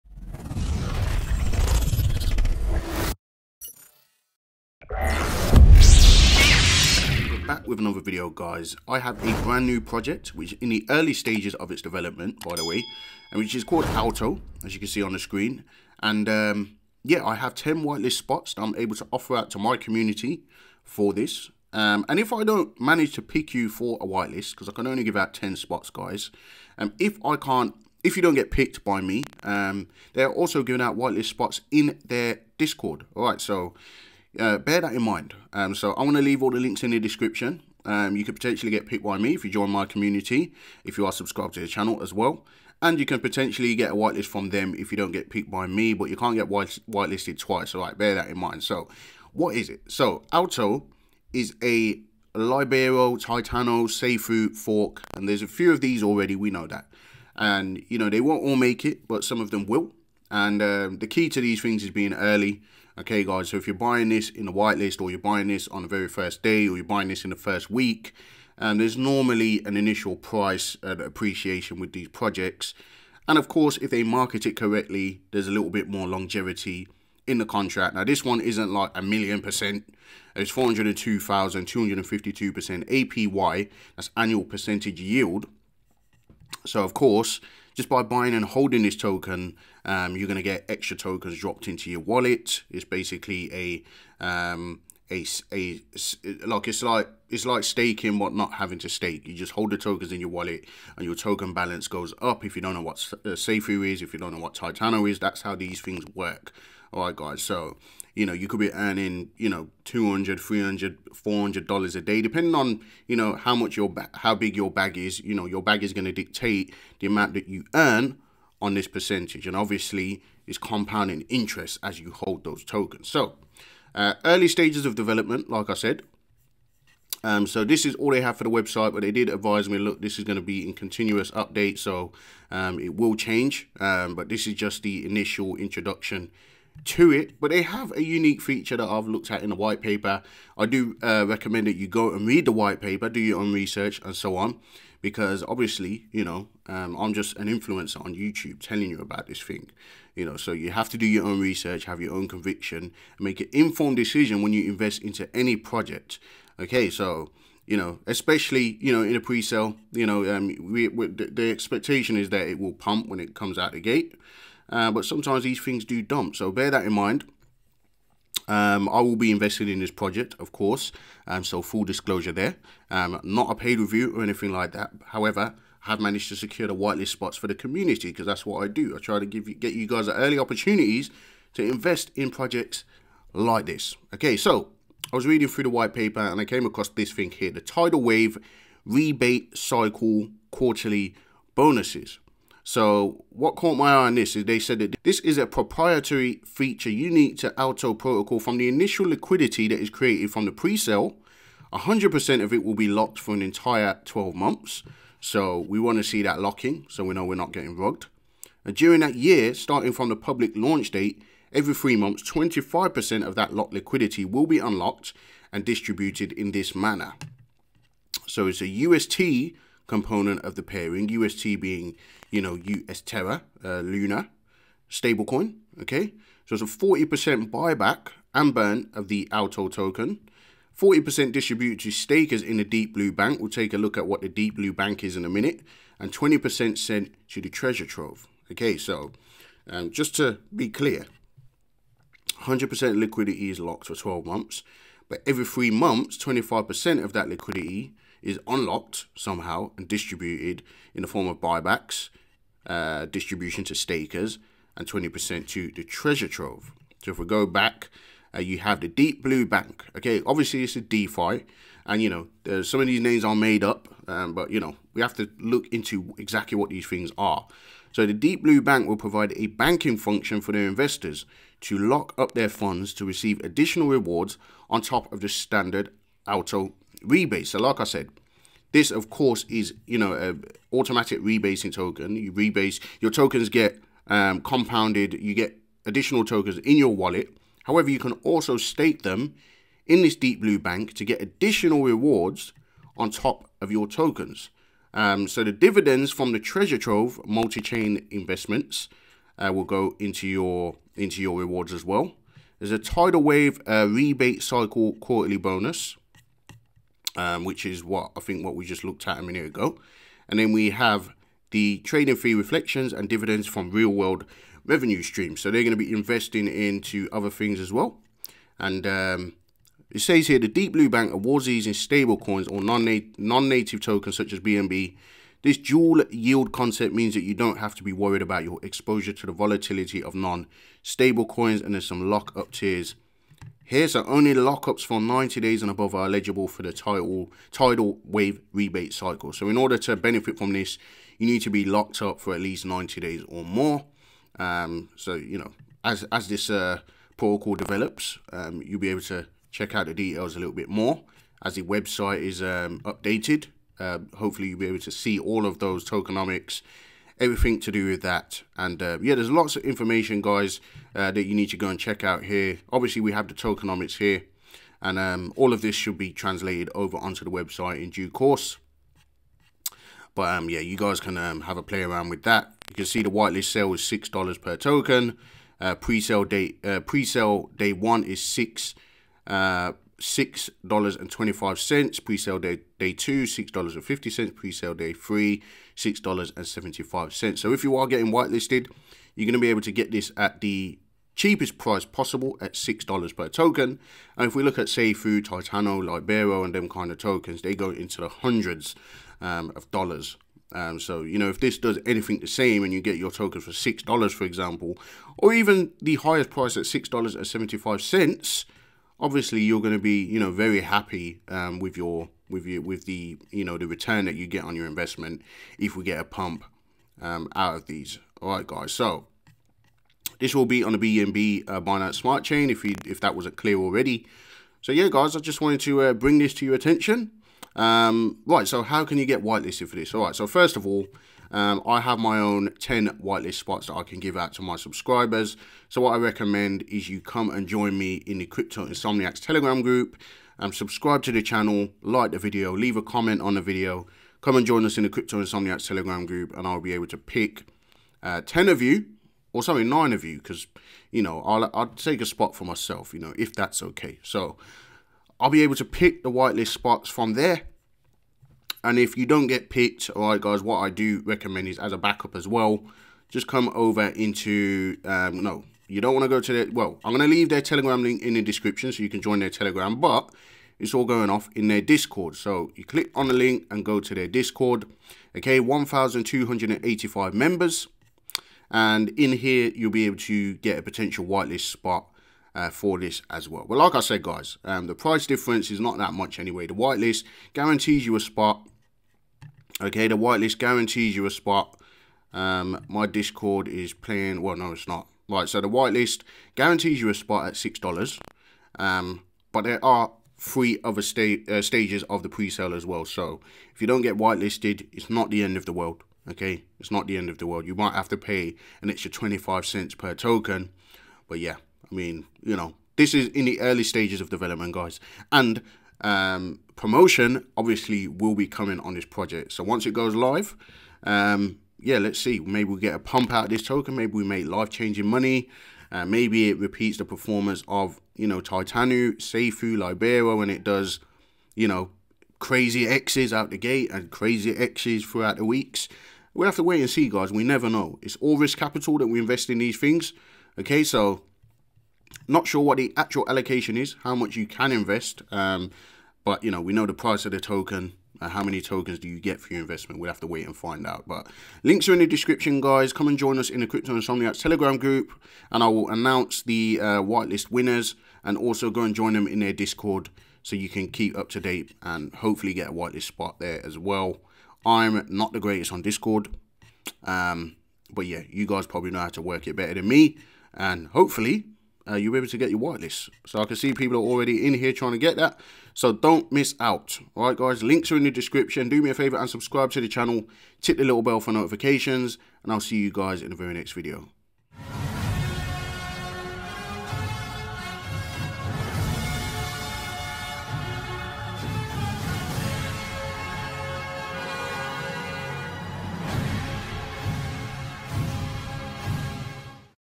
Aalto Protocol 402,252.15 APY Safuu Titano Fork (WHITELIST NOW)
Crypto Insomniacs Official chat by Crypto Lightsaber

Aalto-Protocol is the lates Safuu, Titano & Libero type fork on the binance Smart Chain (BSC). I have 10 whitelist spots given to me by the team which i will be giving out. Find out how in video.